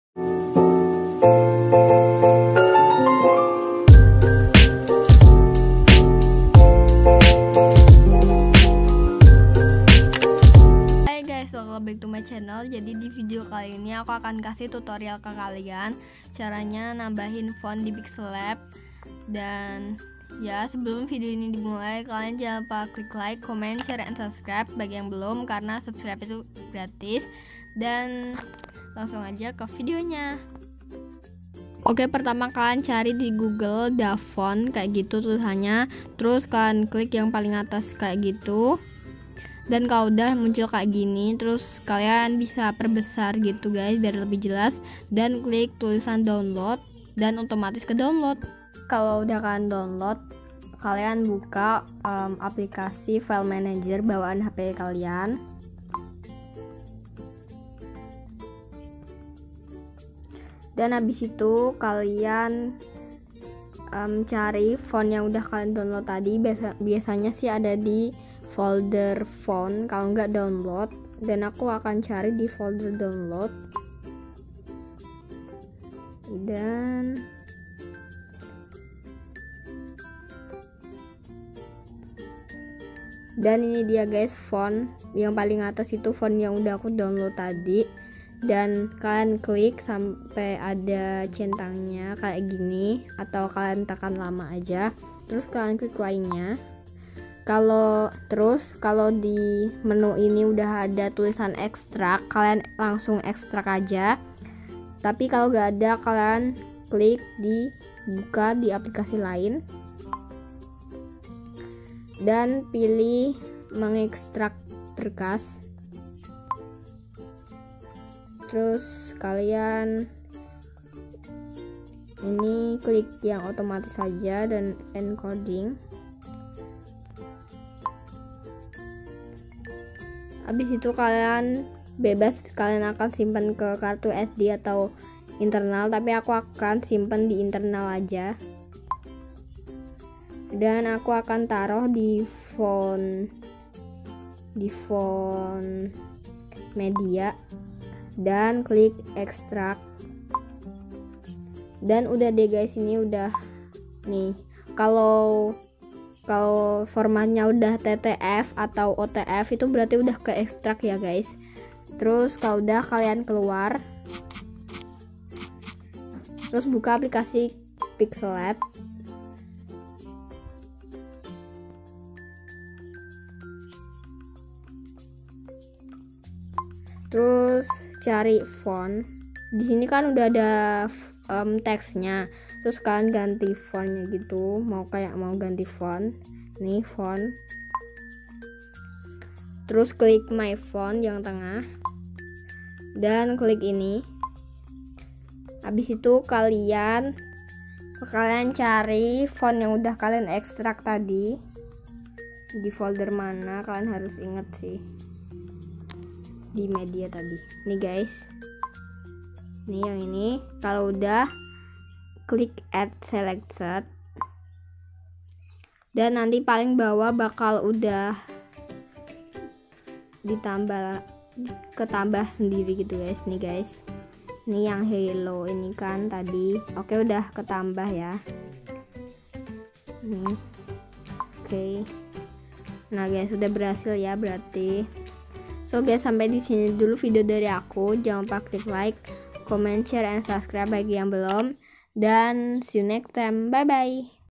CARA MENAMBAHKAN FONT DI PIXELLAB 🧁🍡
CARA MENAMBAH FONT DI PIXELLAB 🧁🍡

Hai guys..kembali lagi dgn video tutorial aku.Jadi di tutorial kali ini aku akan ajarin kalian gimana caranya menambah kan font di pixellab.klo mau tau gimana caranya?? tonton video nya jangan di skip ya 😊!!

🍒Camera : Xiomi redmi 4x
🍒editing video : kinemaster diamond
🍒 Editing thumbnail : pixellab
🍒 screen recorder : X recorder